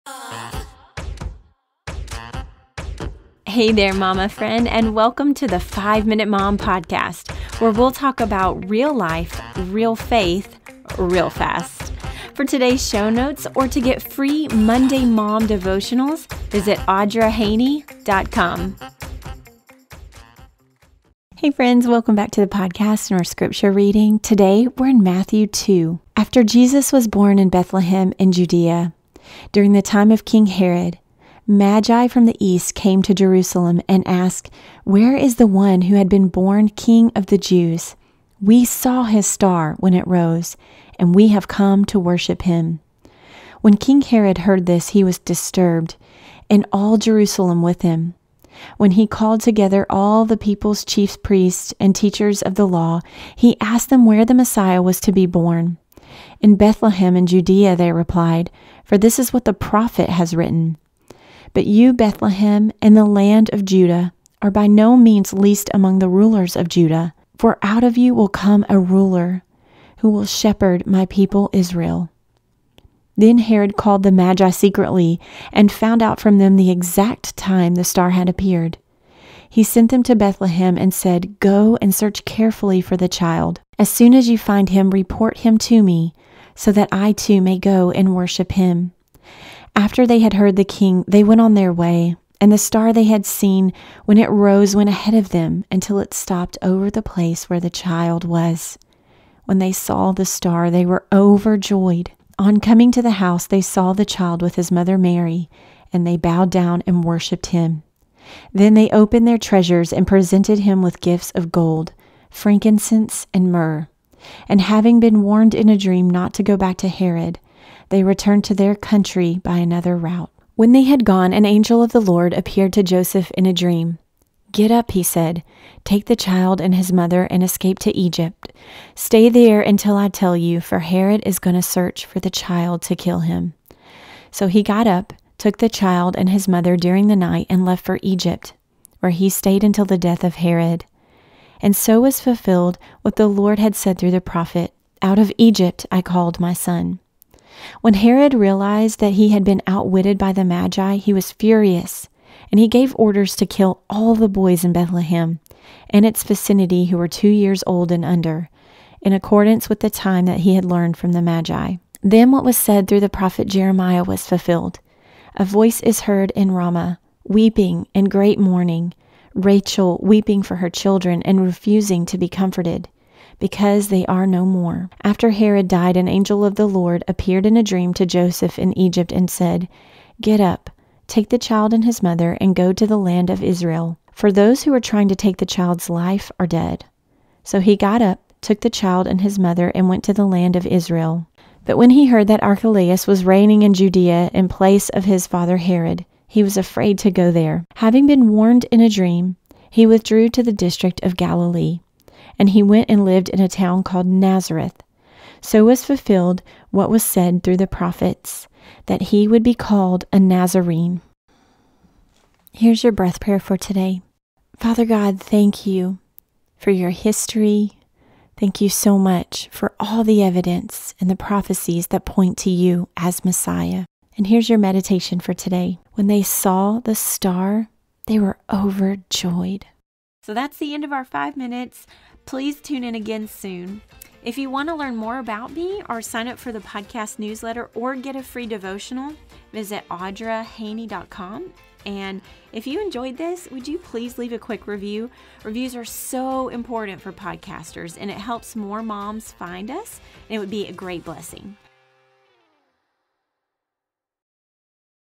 Scripture Series: Matthew 2
Join us for daily scripture reading this fall. In just five minutes a day, we'll be traveling through the gospels together! Scripture reading episodes run Monday-Friday and feature one chapter of the Bible, a quick prayer, and a meditation.